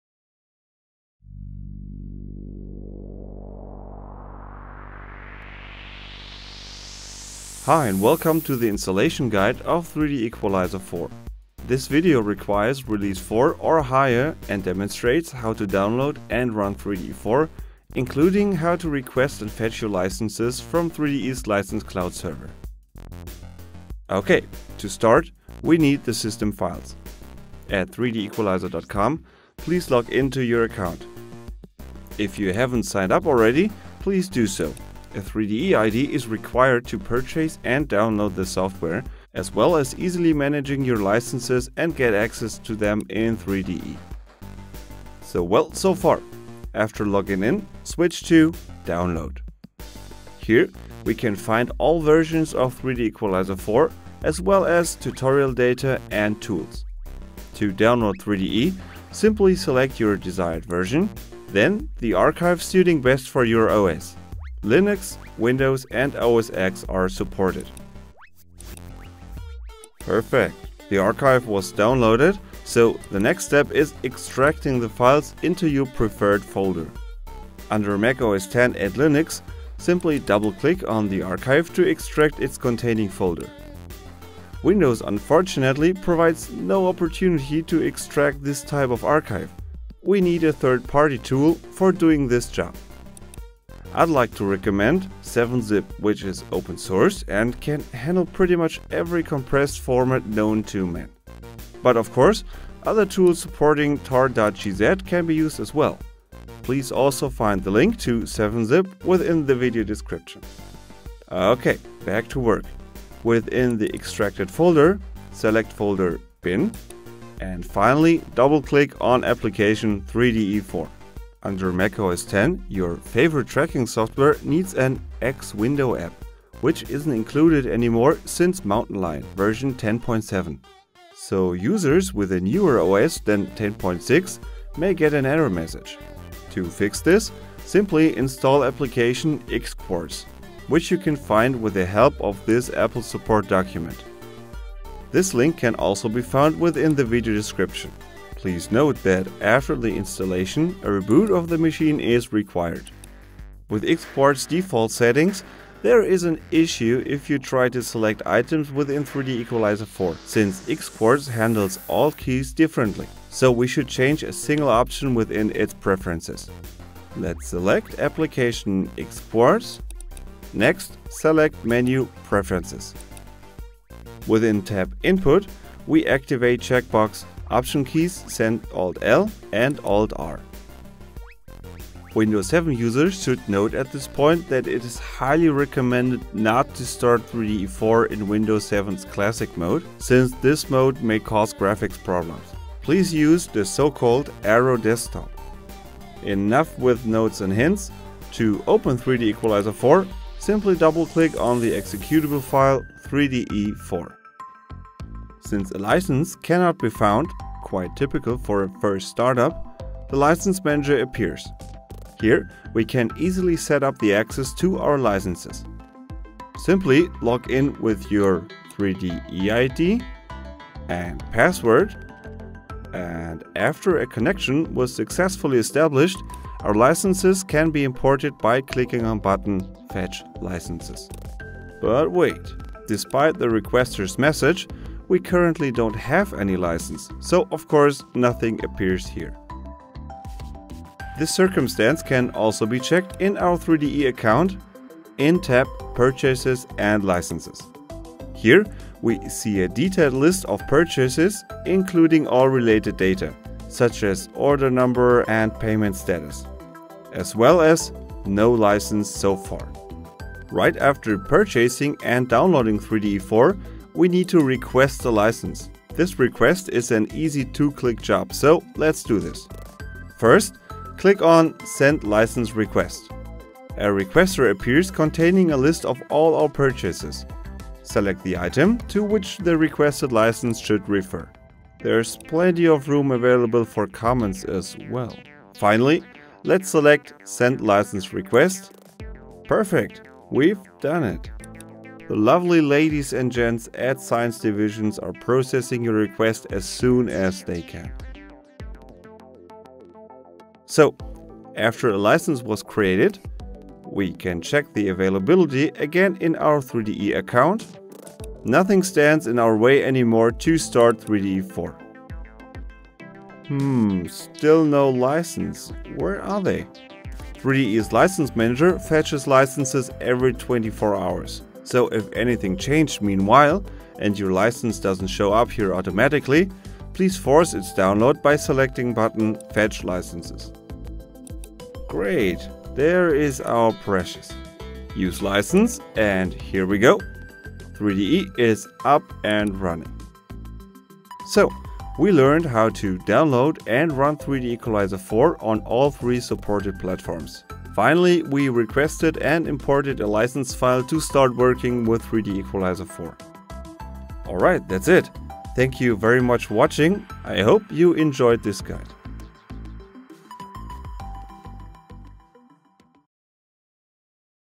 3DEqualizer4 [basic] - Installation Guide 3DE4 Release 4
This guide demonstrates in a few easy steps how to download an run 3DE4 including how to request and fetch licenses from 3DEs license cloud server.

Required version for this guide: 3DEqualizer4 Release 4